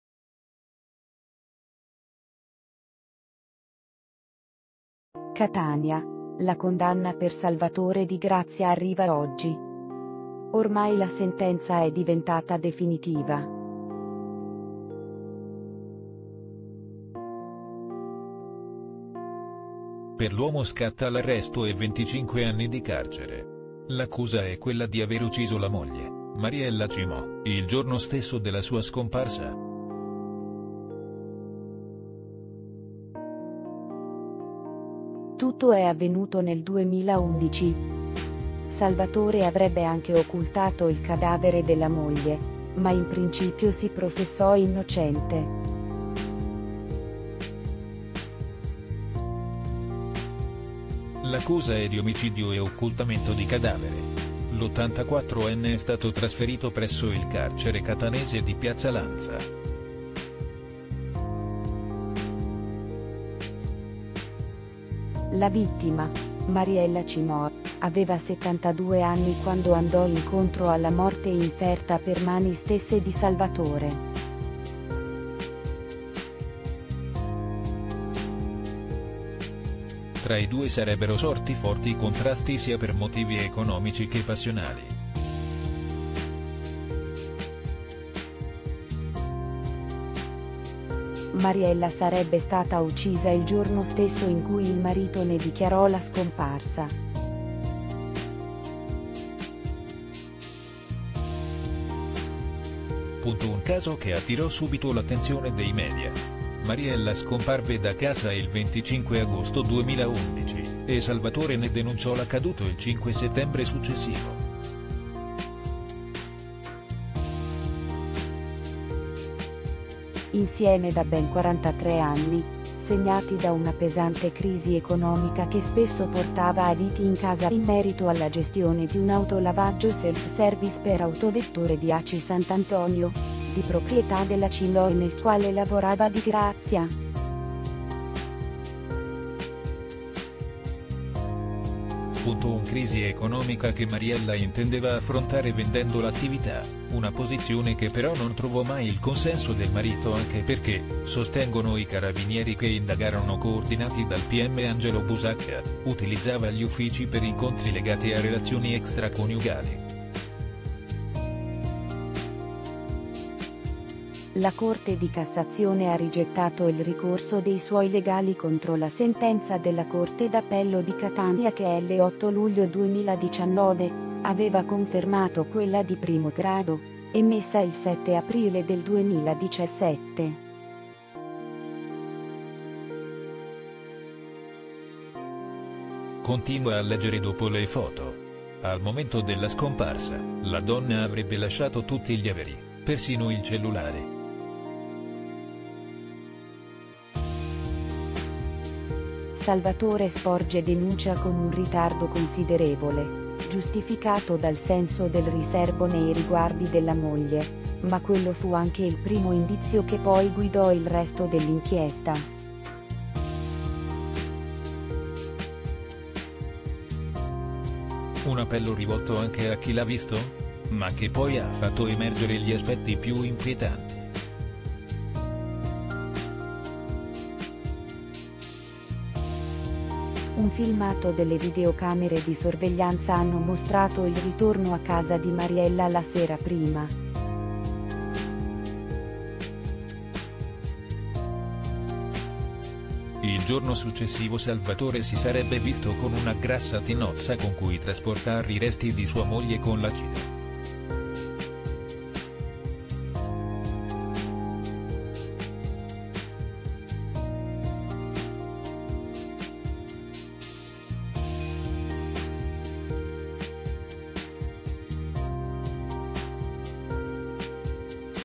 “Arrestato Salvatore Di Grazia”. Dopo 9 anni si chiude così il caso della scomparsa della moglie Mar
Catania, la condanna per Salvatore Di Grazia arriva oggi. Ormai la sentenza è diventata definitiva. Per l’uomo scatta l’arresto e venticinque anni di carcere. L’accusa è quella di aver ucciso la moglie, Mariella Cimò, il giorno stesso della sua scomparsa. Tutto è avvenuto nel 2011. Salvatore avrebbe anche occultato il cadavere della moglie, ma in principio si professò innocente.  L’accusa è di omicidio e occultamento di cadavere. L’84enne è stato trasferito presso il carcere catanese di Piazza Lanza. La vittima, Mariella Cimò, aveva 72 anni quando andò incontro alla morte inferta per mani stesse di Salvatore. Tra i due sarebbero sorti forti contrasti sia per motivi economici che passionali. Mariella sarebbe stata uccisa il giorno stesso in cui il marito ne dichiarò la scomparsa. ().        Un caso che attirò subito l’attenzione dei media. Mariella scomparve da casa il  25 agosto 2011, e Salvatore ne denunciò l’accaduto il 5 settembre successivo. Insieme da ben 43 anni, segnati da una pesante crisi economica che spesso portava a liti in casa in merito alla gestione di un autolavaggio self service per autovetture di Aci Sant’Antonio, di proprietà della Cimò e nel quale lavorava Di Grazia. ().       Un crisi economica che Mariella intendeva affrontare vendendo l’attività, una posizione che però non trovò mai il consenso del marito anche perché, sostengono i carabinieri che indagarono coordinati dal Pm Angelo Busacca, “utilizzava gli uffici per incontri legati a relazioni extraconiugali”. La Corte di cassazione ha rigettato il ricorso dei suoi legali contro la sentenza della Corte d’appello di Catania che l’8 luglio 2019, aveva confermato quella di primo grado, emessa il 7 aprile del 2017. (Continua a leggere dopo le foto).      Al momento della scomparsa, la donna avrebbe lasciato tutti gli averi, persino il cellulare. Salvatore sporge denuncia con un ritardo considerevole, giustificato dal senso del riserbo nei riguardi della moglie, ma quello fu anche il primo indizio che poi guidò il resto dell’inchiesta. Un appello rivolto anche a Chi l’ha visto? ma che poi ha fatto emergere gli aspetti più inquietanti. Un filmato delle videocamere di sorveglianza hanno mostrato il ritorno a casa di Mariella la sera prima. Il giorno successivo Salvatore si sarebbe visto con una grassa tinozza con cui trasportar i resti di sua moglie con l’acido.